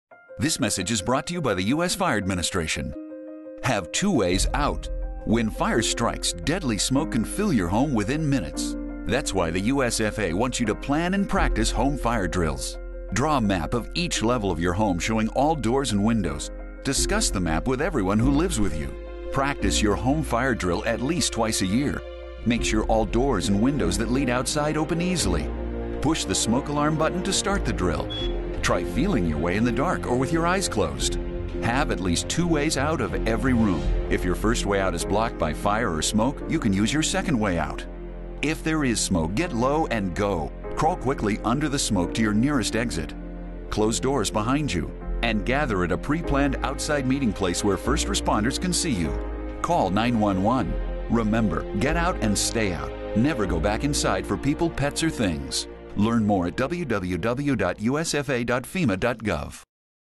Fire Safety - Have Two Ways Out
Plan and practice home fire drills at least twice a year. Make sure you know two ways out of each room.